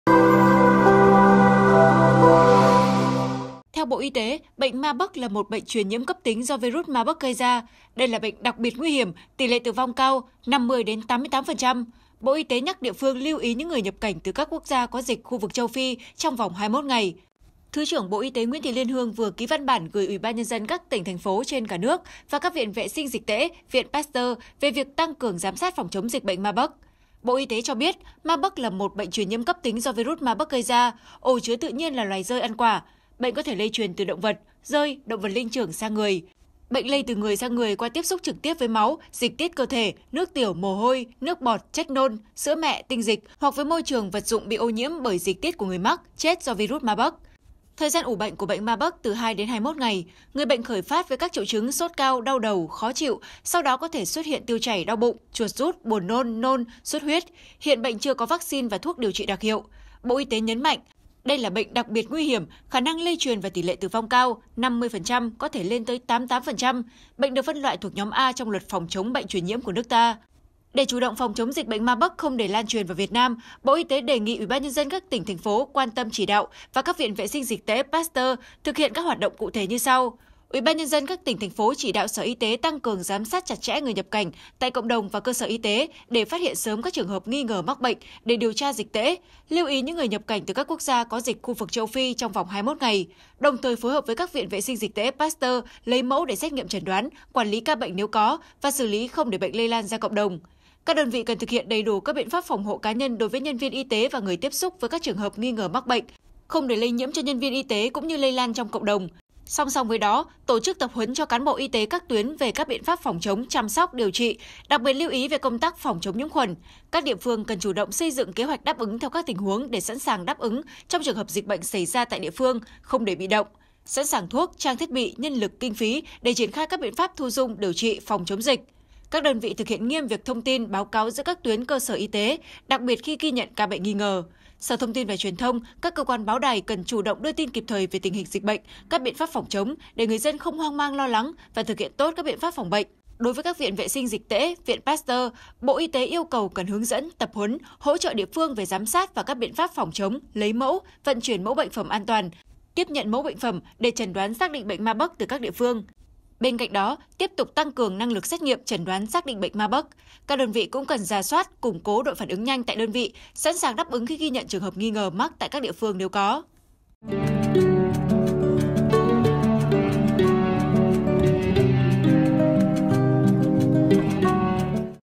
Bộ Y Tế Yêu Cầu Giám Sát Chặt, Phòng Căn Bệnh Truyền Nhiễm Đặc Biệt Nguy Hiểm Xâm Nhập | SKĐS
SKĐS | Theo Bộ Y tế, bệnh Marburg là một bệnh truyền nhiễm cấp tính do virus Marburg gây ra. Đây là bệnh đặc biệt nguy hiểm, tỷ lệ tử vong cao, 50-88%. Bộ Y tế nhắc địa phương lưu ý những người nhập cảnh từ các quốc gia có dịch khu vực châu Phi trong vòng 21 ngày.

Mời quý vị và các bạn xem thêm: Nam Cảnh Sát PCCC Hiến Máu Cứu Sản Phụ Qua Cơn Nguy Kịch